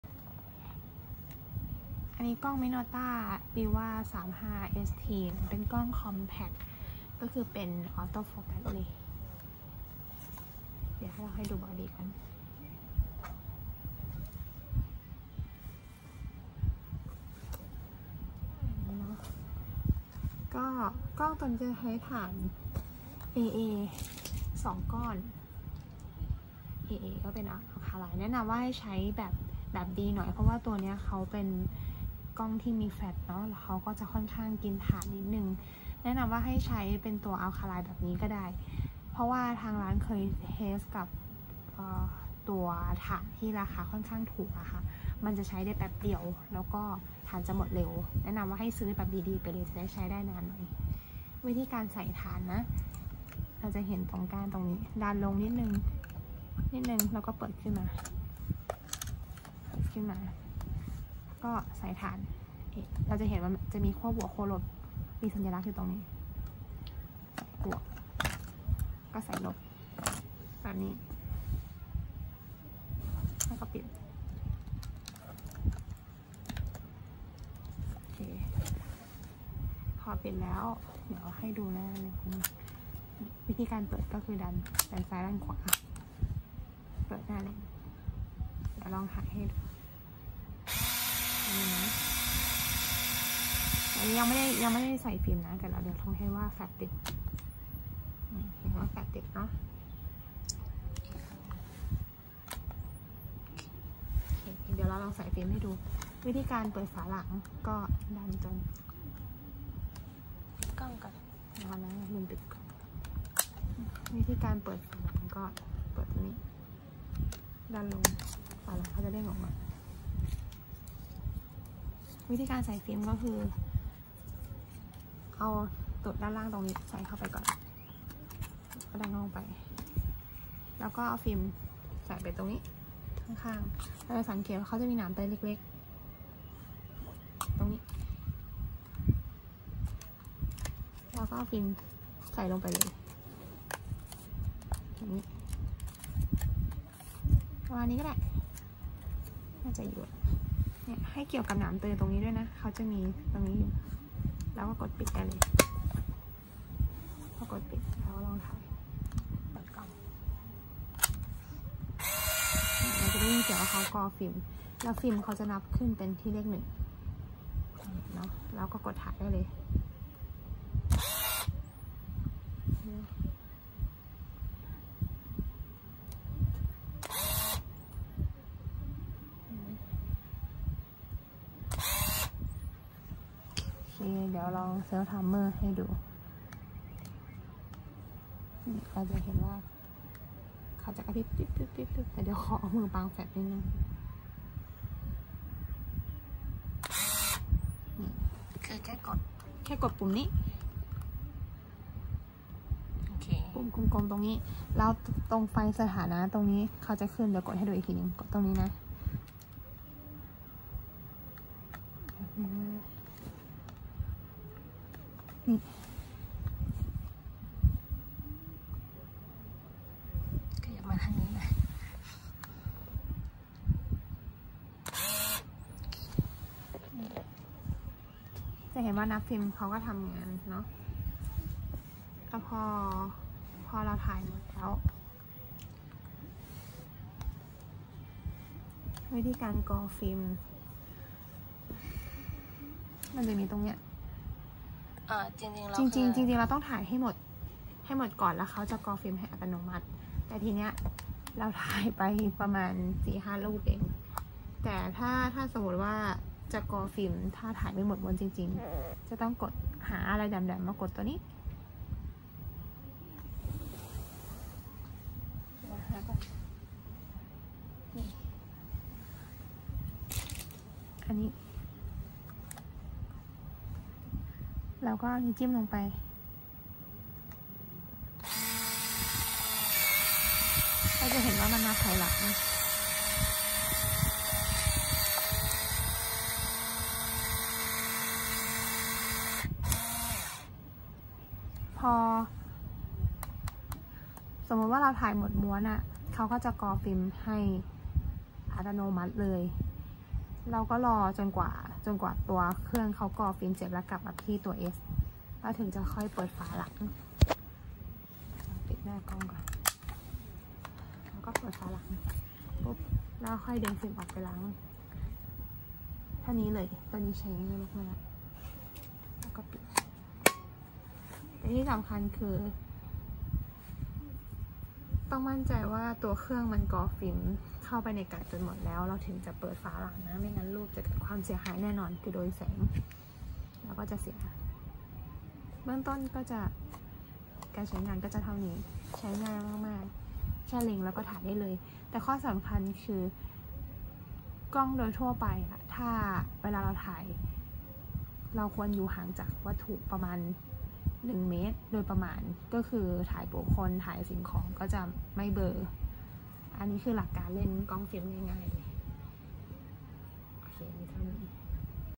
[Review] Minolta Riva 35st compact camera by onemoreshotstore
A brief video tutorial on using Minolta Riva 35st